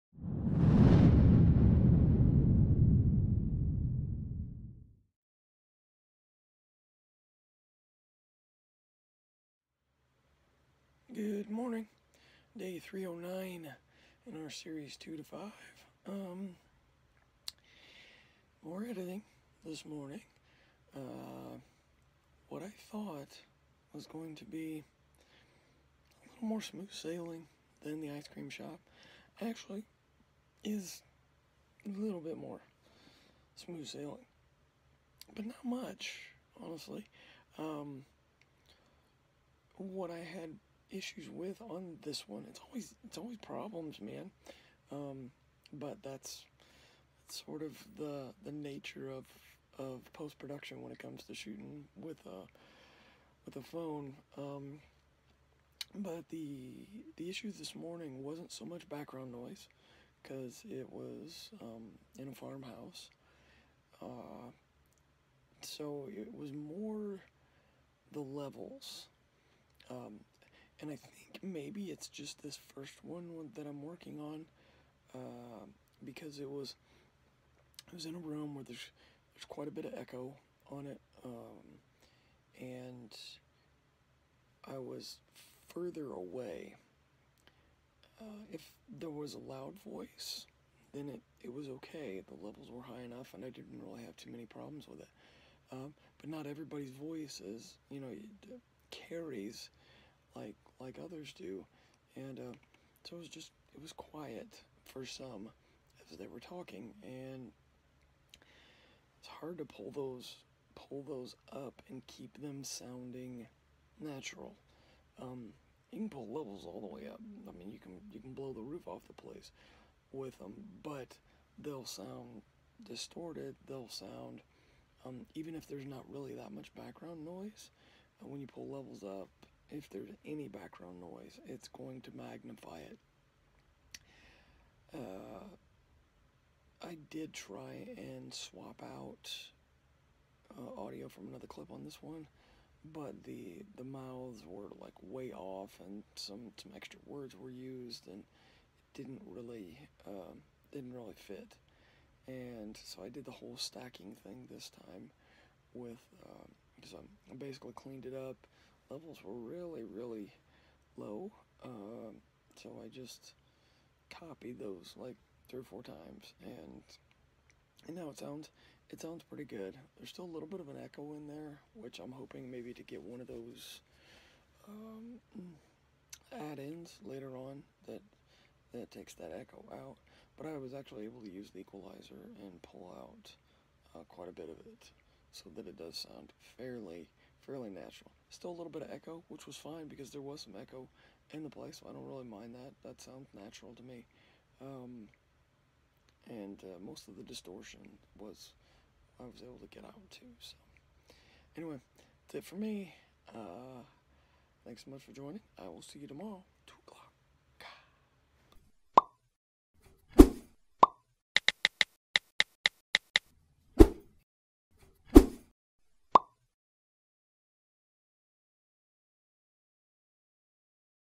2 To 5 "Only A Little" Ep. 309
Filmmaker Kevin L. Mounce shares a look into his creative editing and writing process from 2-5am every morning.